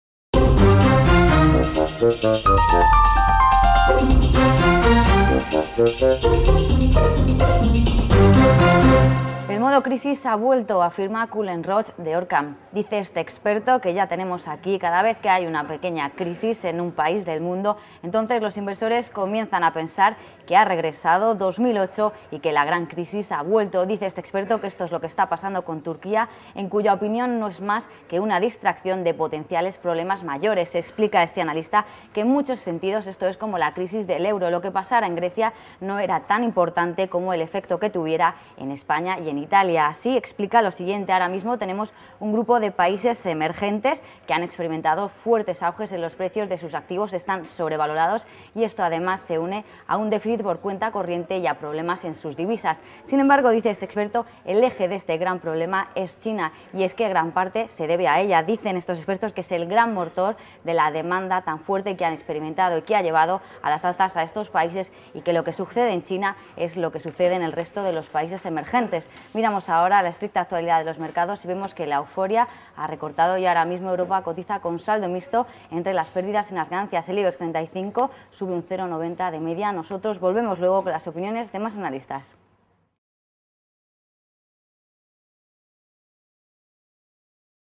Con lupa
Cullen Roche, fundador de Orcam Financial Group, afirma que Turquía "es una distracción en medio de problemas (potenciales) mucho mayores". Considera este experto que "parece que hemos vuelto al 'modo crisis'". La clave para este analista es lo que suceda en China.